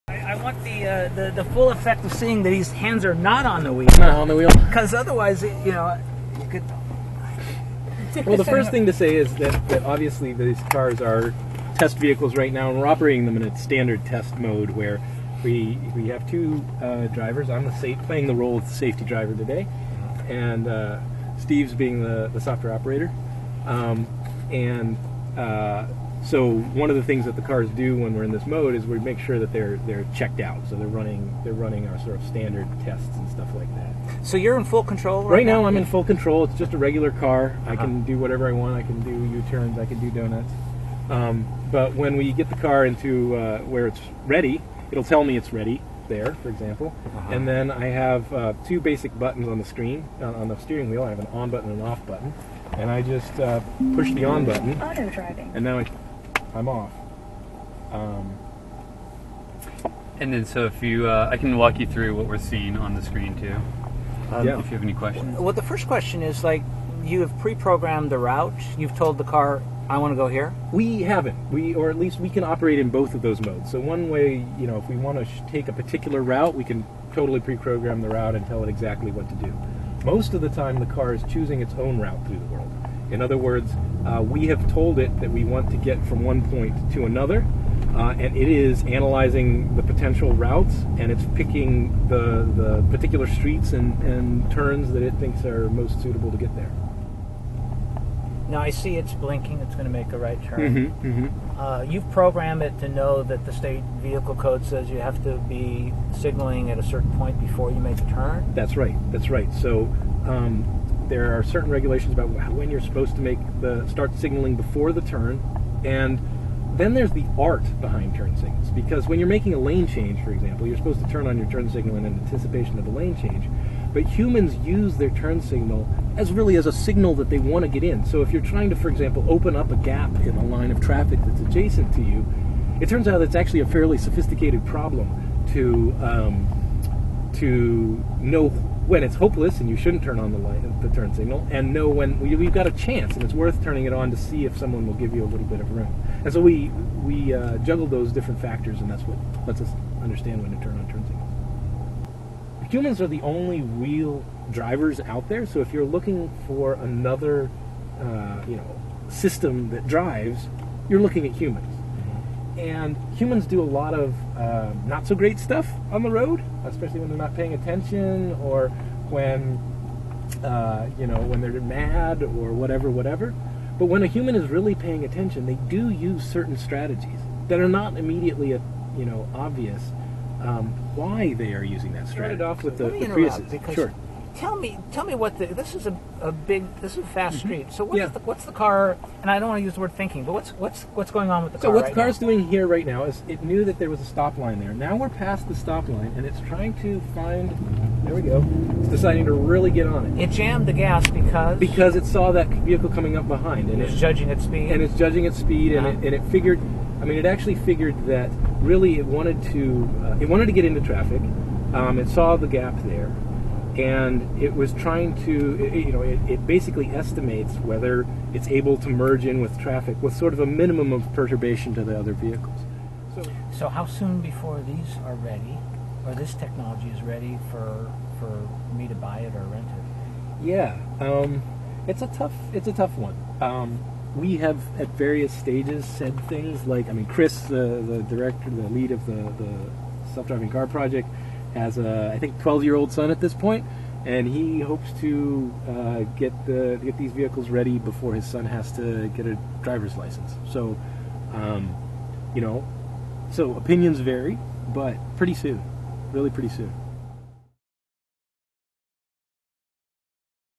A 360 view inside Google's self-driving car
The Sacramento Bee's Tony Bizjak takes a ride in Google's self-driving car.